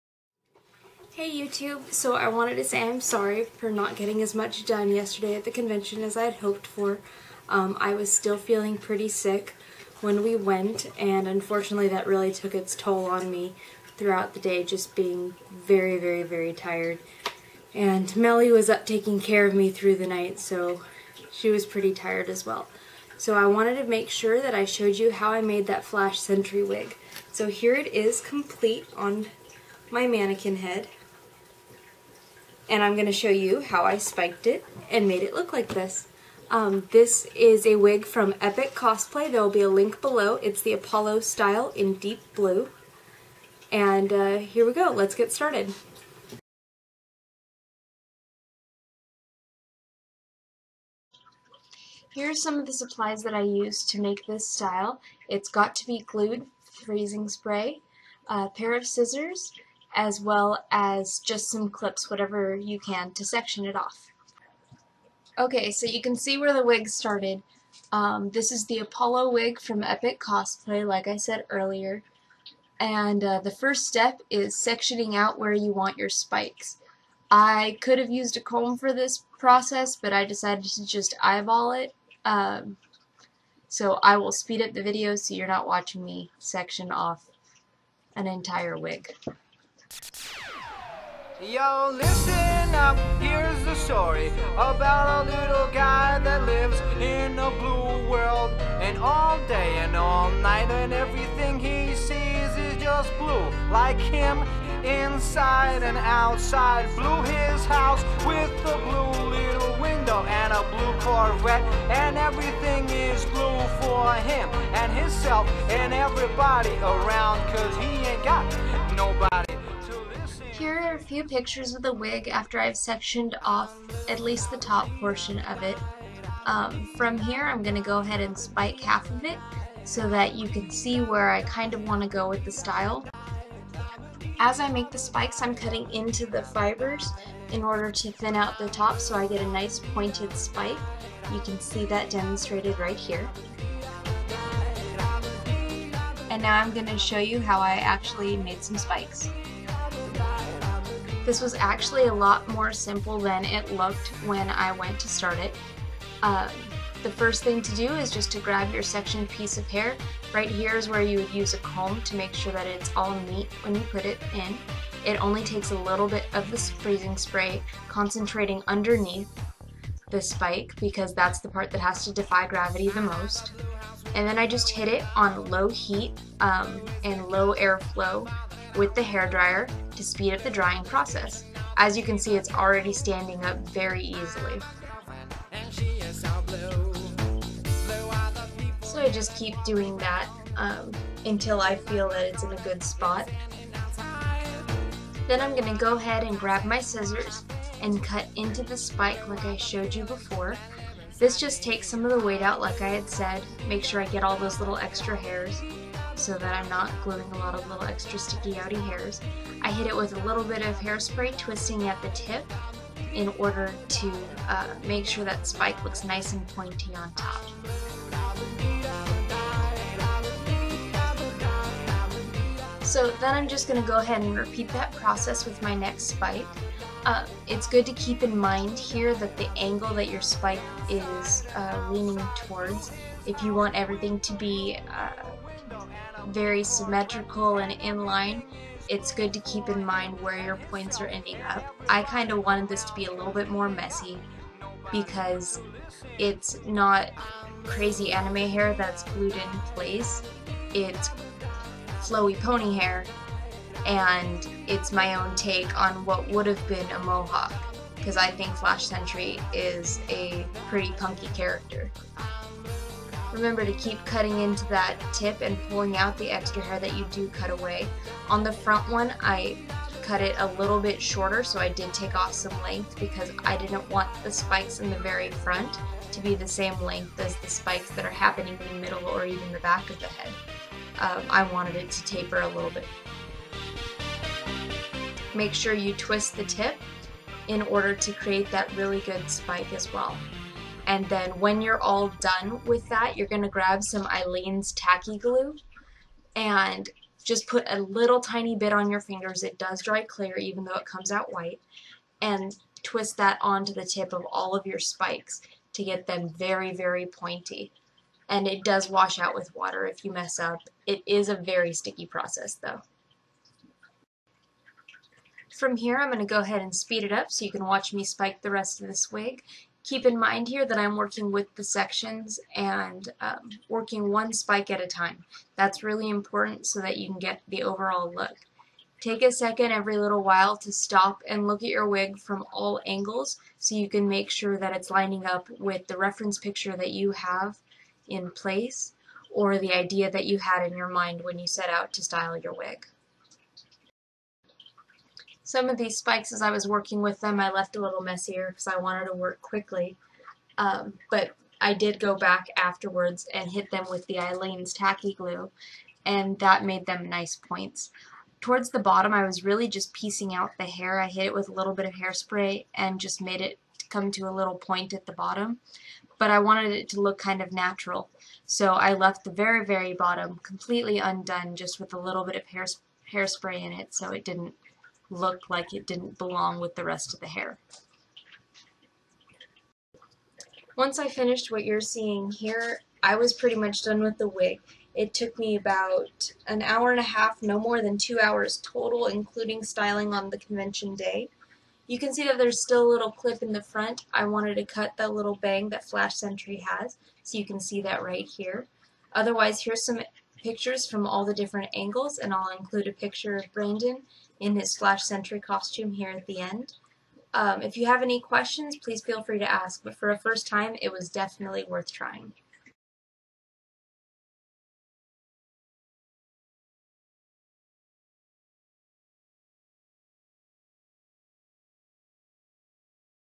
How to Spike the Apollo Wig
This is a quick video on how I styled the Apollo wig by Epic Cosplay.

Materials needed:
Got2b Freeze Spray
Aileen's Tacky Glue
Clips or bobby pins
Blow Dryer (used on low heat, low flow setting)

Happy Cosplaying~